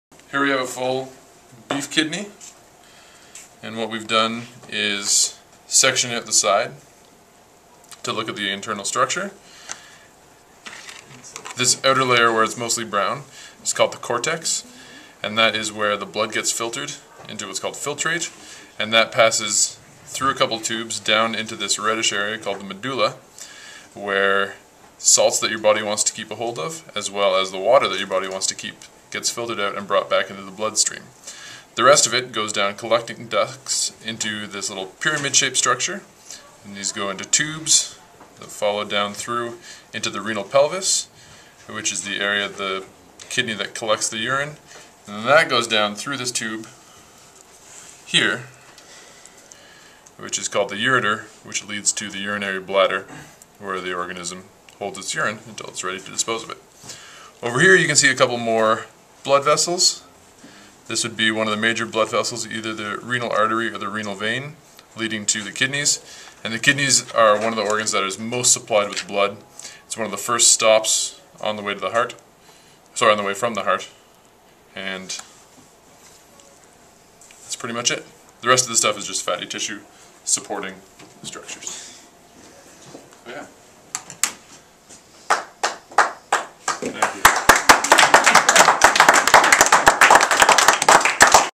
Ridley College AP Biology Class Kidney Dissection
Mr. Bowen explains the functions of the Kidney through a beef kidney dissection in the Advanced Placement Biology Class at Ridley College.